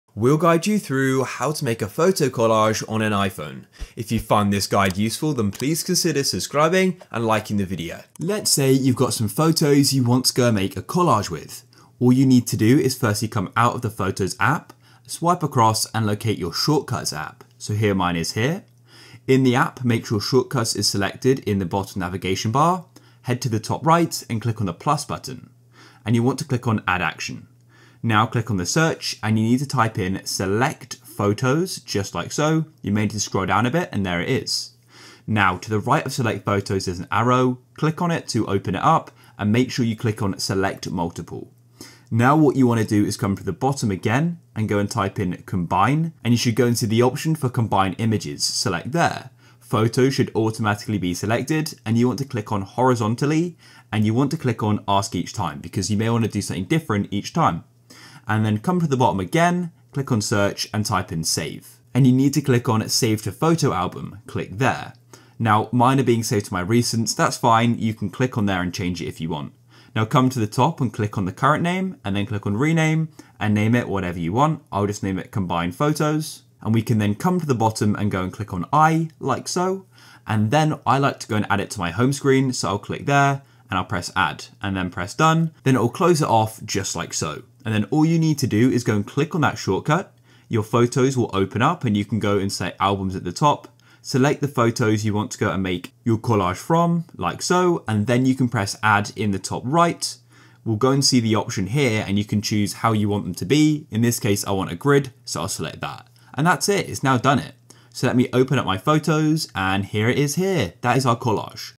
How To Make Photo Collage On iPhone (No Apps) - Full Guide
Learn how to make photo collage on iphone in this video.

GuideRealm